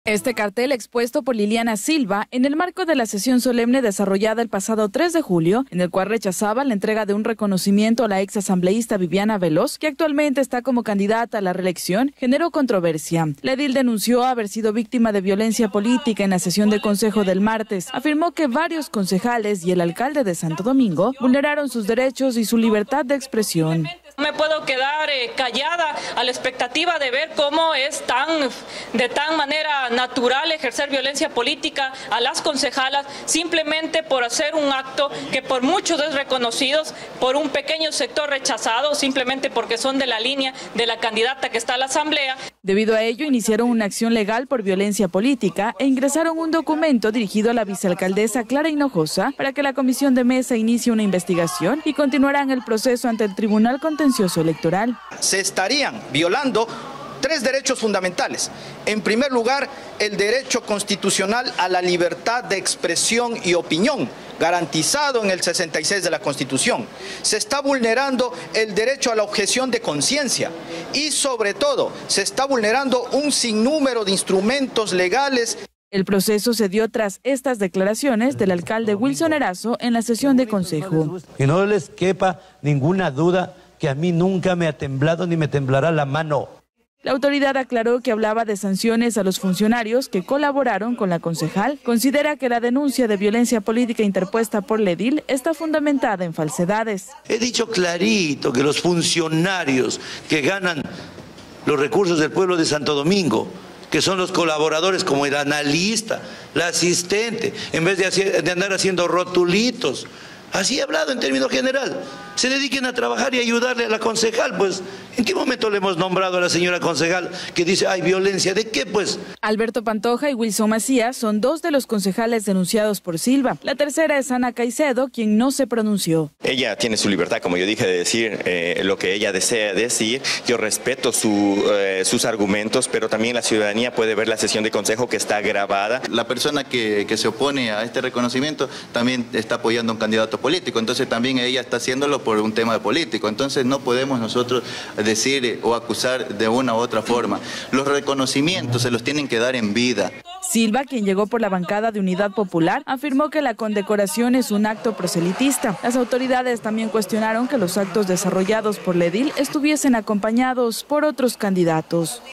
La concejal Liliana Silva denunció haber sido víctima de violencia política
En rueda de prensa, la concejal Liliana Silva denunció haber sido víctima de violencia política en el pleno del concejo municipal por tres concejales y el alcalde de Santo Domingo. Todo esto se dio tras el cartel presentado por la edil en la sesión solemne, en el cual rechazaba la condecoración a la exasambleísta Viviana Veloz.